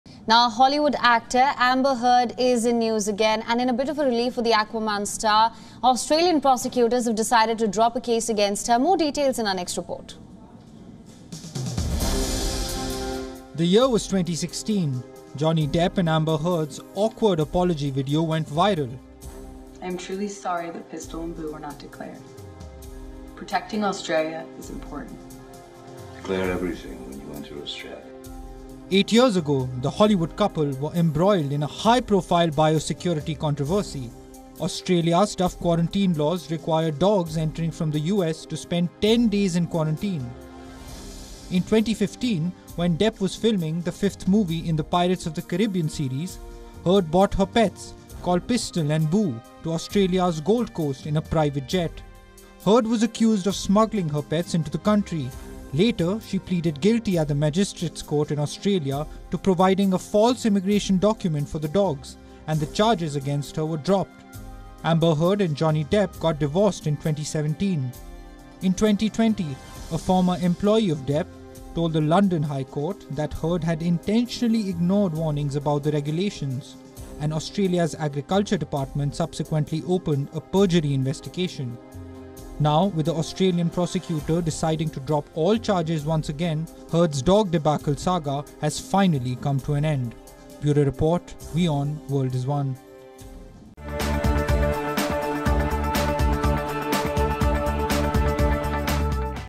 Australia prosecutors drop Amber Heard perjury case over smuggling of dogs Pistol, Boo | World DNA
According to the government, Australian prosecutors withdrew a prospective criminal case against American actor Amber Heard over claims that she misled a judge about how her Yorkshire terriers Pistol and Boo entered Australia eight years ago.